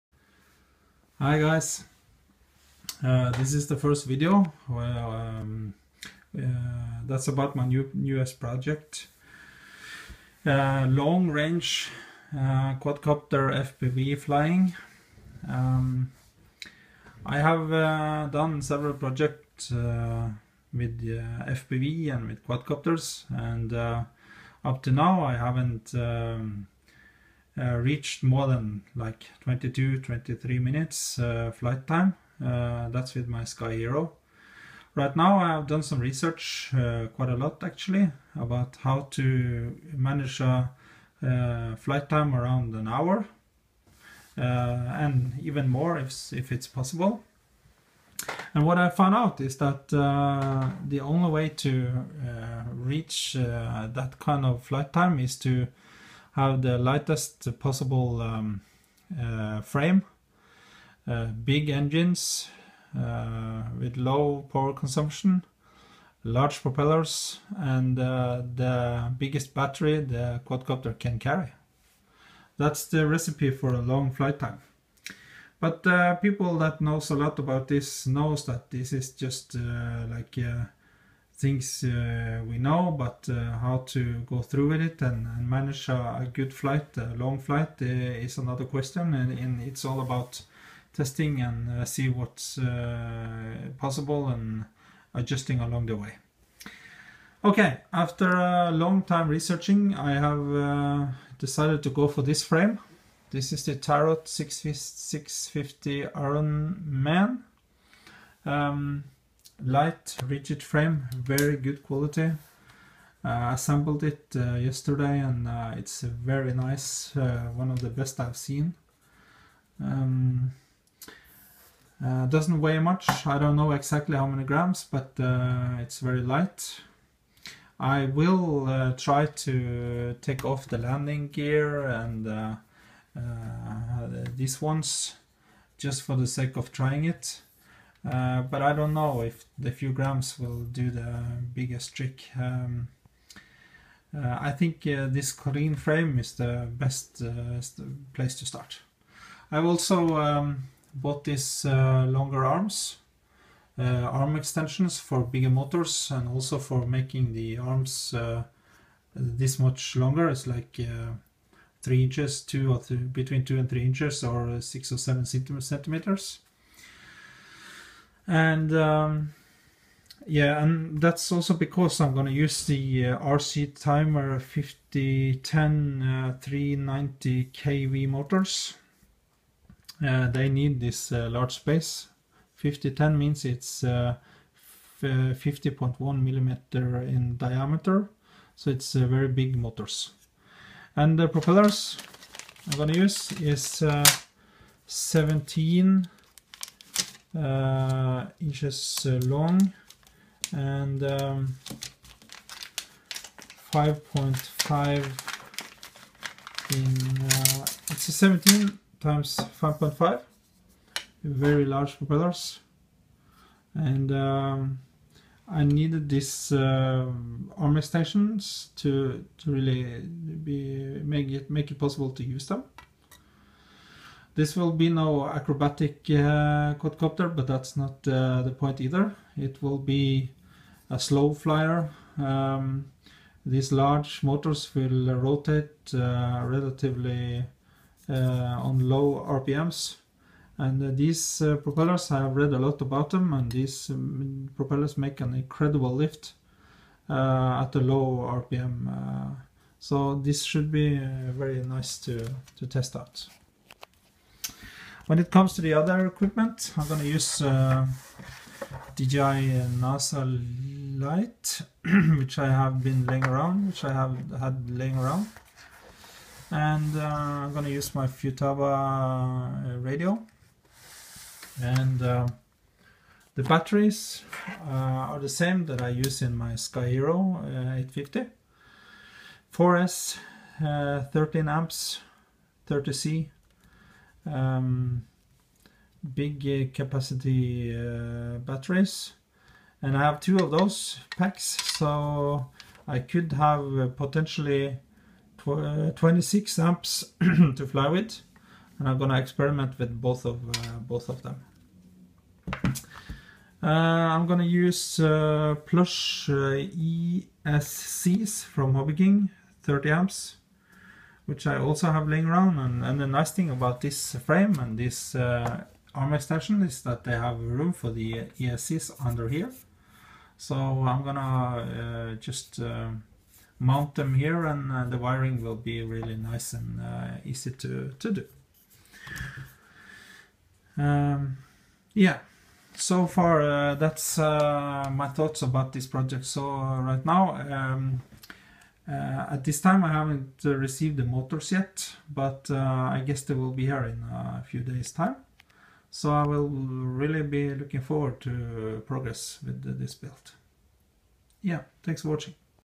60-80 min quadcopter flight project Part 1
I've been researching a lot for a setup for flying 60 minutes+. This is what I have found out so far.